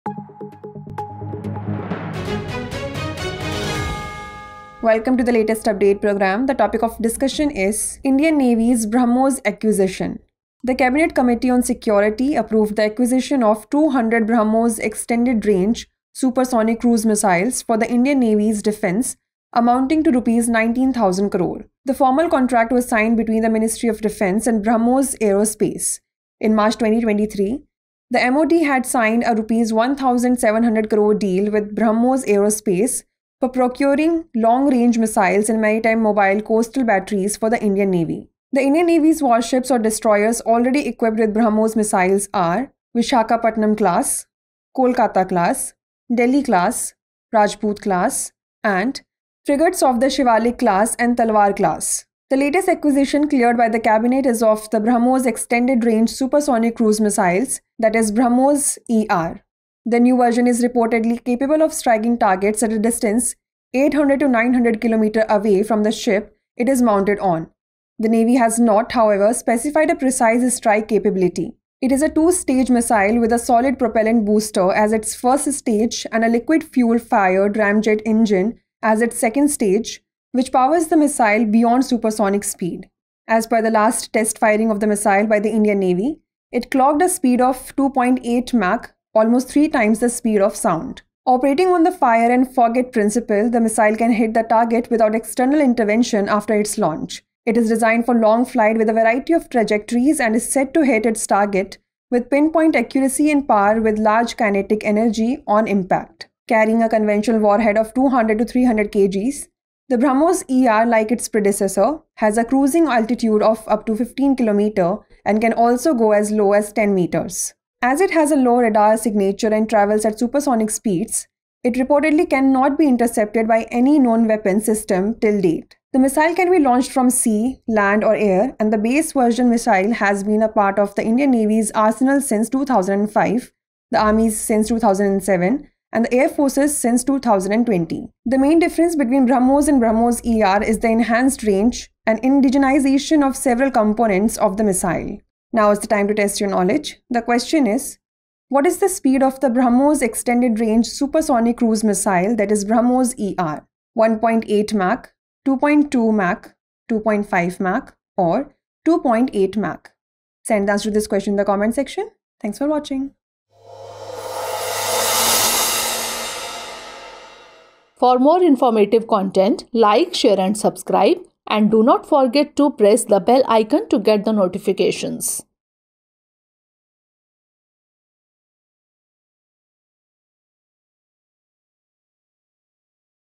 Indian Navy's BrahMos Acquisition | Latest update | Drishti IAS English
The Indian Navy strengthens its defense arsenal with the approval of a ₹19,000-crore acquisition of 200 BrahMos Extended Range supersonic cruise missiles, enhancing its surface-to-surface capabilities.